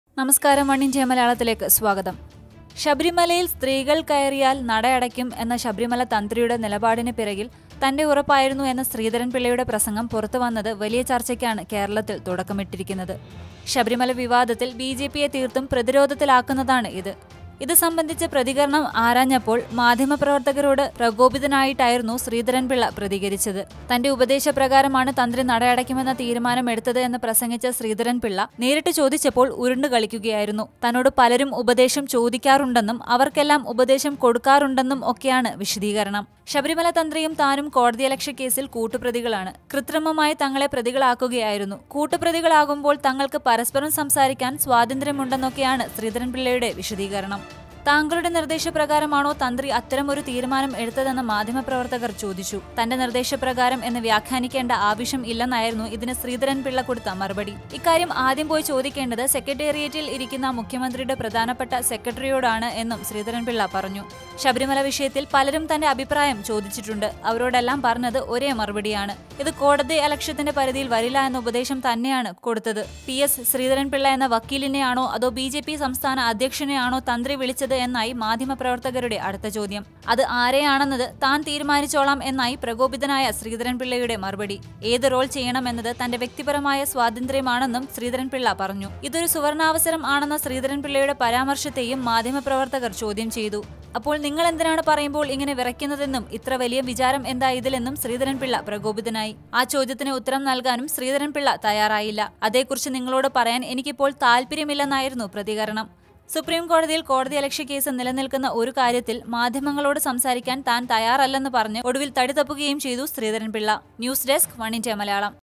വിവാദ പ്രസംഗം പുറത്ത് വന്നപ്പോള്‍ ഉരുണ്ടുകളിച്ച് ശ്രീധരന്‍ പിള്ള | Oneindia malayalam
PS Sreedharan Pillai's reaction on leaked controversial speech
ശബരിമല വിഷയത്തില്‍ പലരും തന്റെ അഭിപ്രായം ചോദിച്ചിട്ടുണ്ട്. അവരോടെല്ലാം പറഞ്ഞത് ഒരേ മറുപടിയാണ്. ഇത് കോടതിയലക്ഷ്യത്തിന്റെ പരിധിയില്‍ വരില്ല എന്ന ഉപദേശം തന്നെ ആണ് കൊടുത്തത്.